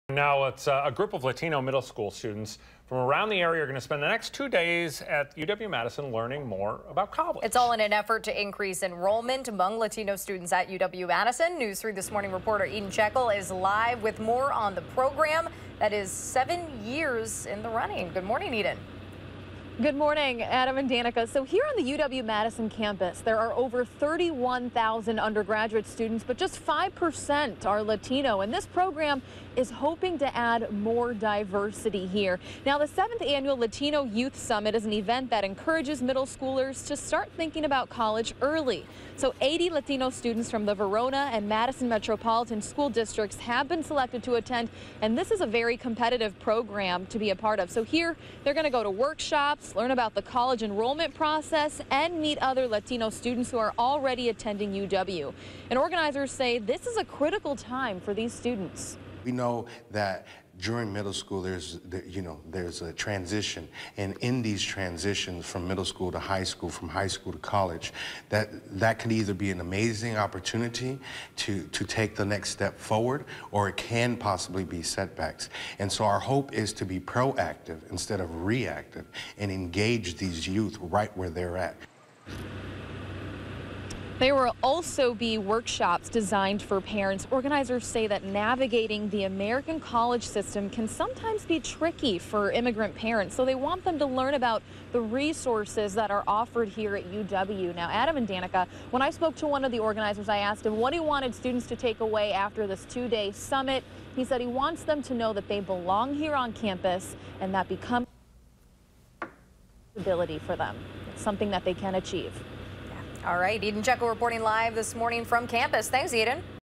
Summit aims to increase Latino enrollment at UW-Madison
UW-Madison wants to increase diversity in its enrollment by inviting Latino students to campus Wednesday and Thursday.
Eden Checkol reports.